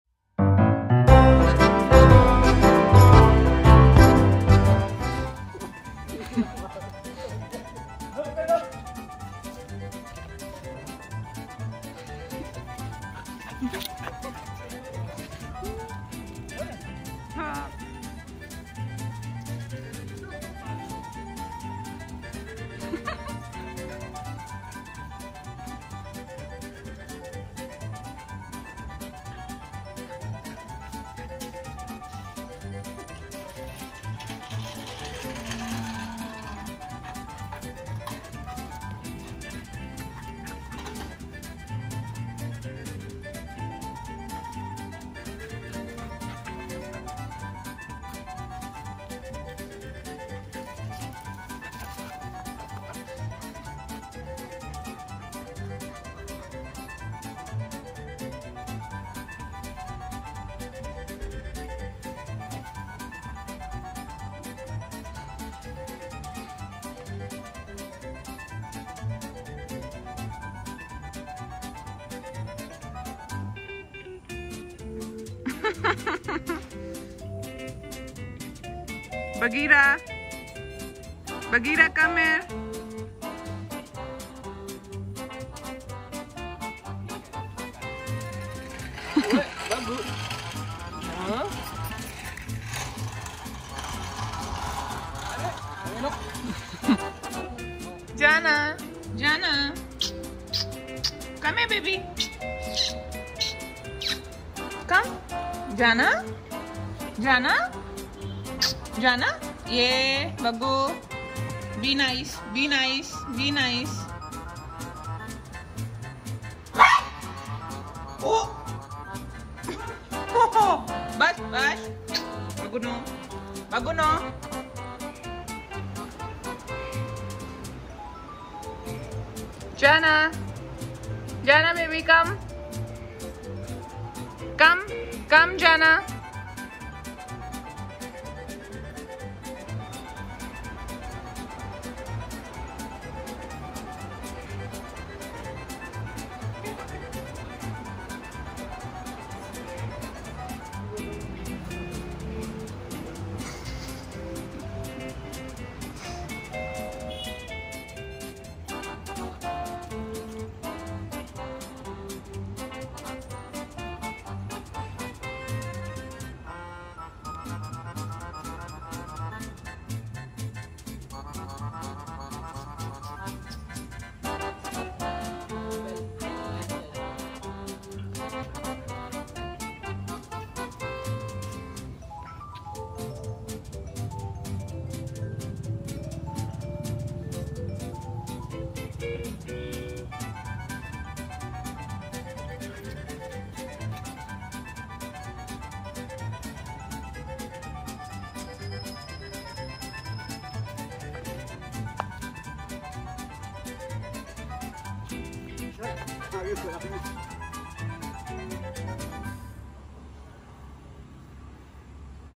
Super Excited Puppy meets his Girlfriend ||Golden Retriever meets Lhasa Apso|| 2020
Our little Golden Retriever puppy Bagheera welcomes his neighbouring Lhasa Apso puppy Jana. He meets the new puppy for the first time. Although, Bagheera is so excited and more than willing to play with her all the time but Jana seemed a bit disinterested in the alien surroundings. She even barked on Bagheera. Ohh, the poor pup Bagheera, continues on his journey!!

Bagheera
KCI Golden Retriever Puppy
Being cute since 12th June 2020